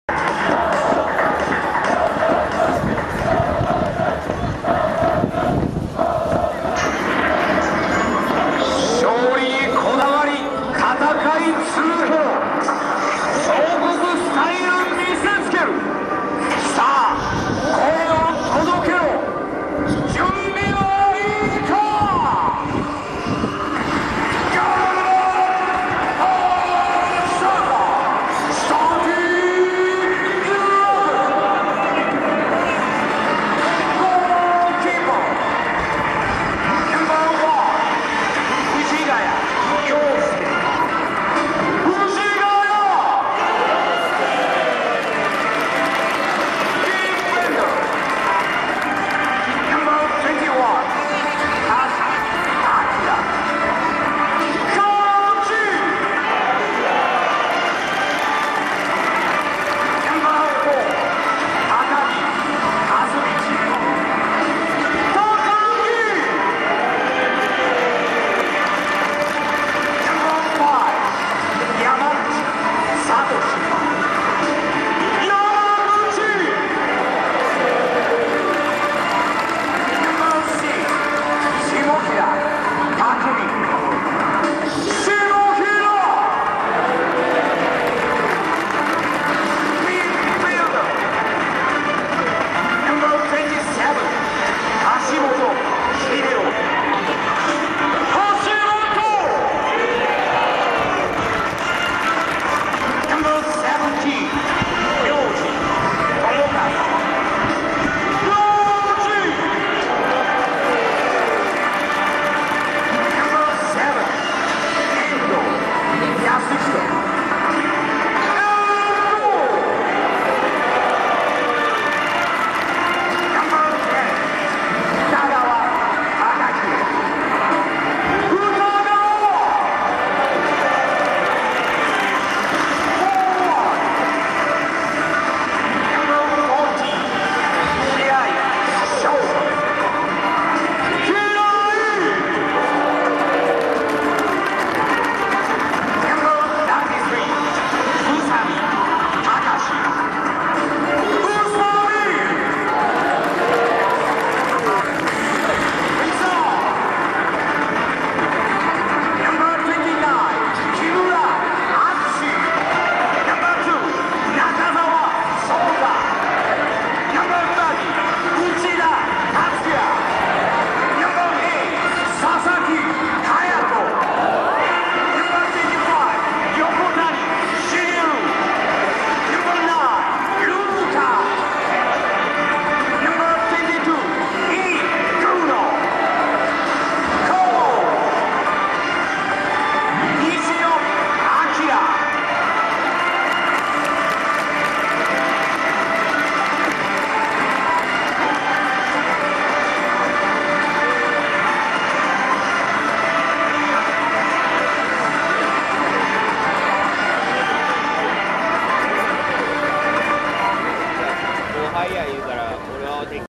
2010年11月23日ガンバ大阪vs湘南ベルマーレ ガンバのスタメン紹介
2010年11月23日に万博スタジアムで開催されたガンバ大阪vs湘南ベルマーレ戦のガンバ大阪のスターティングイレブンの紹介です。